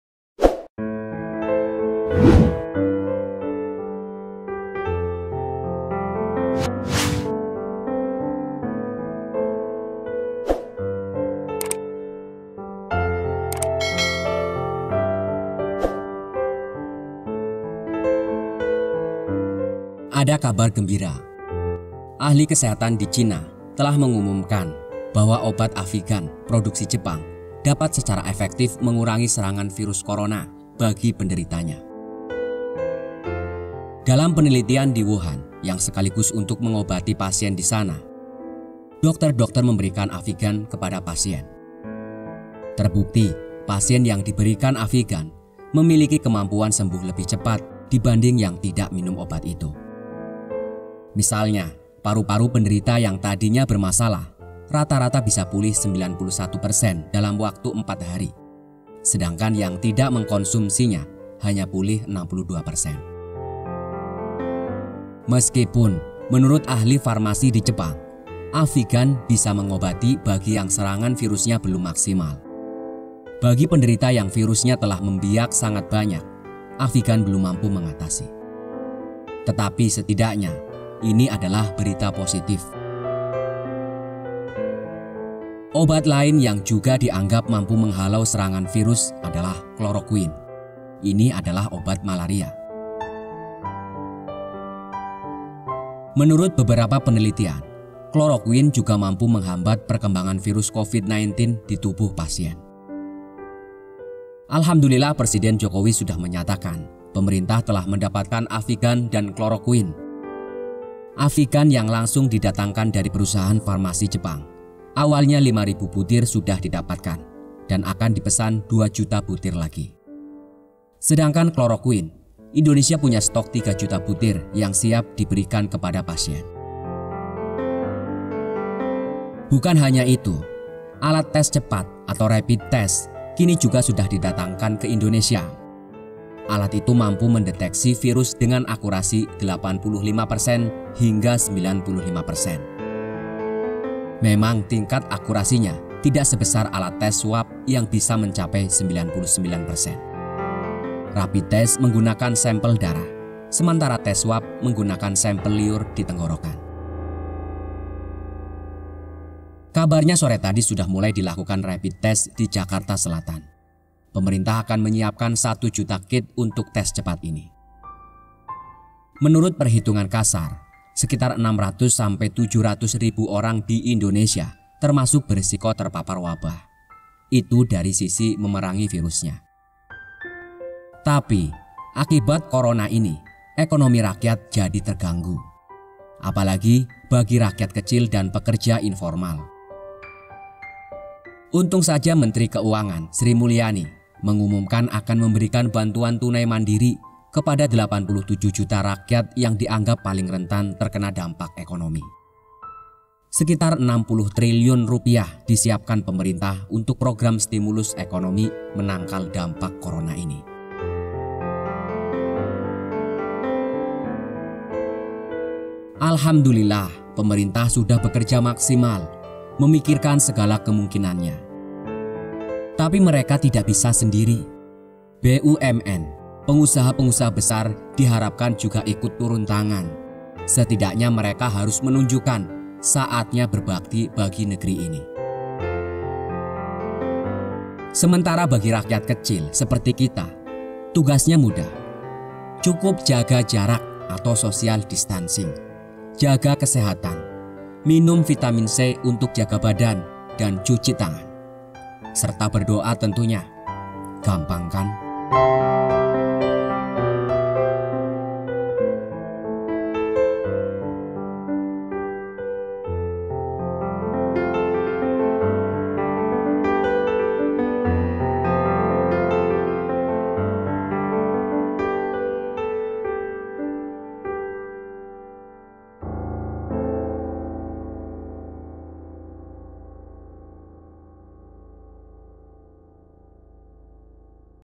Tugas Kita Cuma Jaga Jarak, Jaga Badan, Cuci Tangan Dan Doa
Pemerintah sudah bekerja maksimal. Memikirkan segala kemungkinannya. Tapi mereka gak bisa sendiri. BUMN, Pengusaha-pengusaha besar diharapkan juga ikut turun tangan. Setidaknya mereka harus menunjukan saatnya berbakti bagi negeri ini.

Channel Melawan Miskin berisi video visualisasi/narasi dari artikel pilihan kami.
Sepenuhnya merupakan opini/berita dari penulis yang bersangkutan.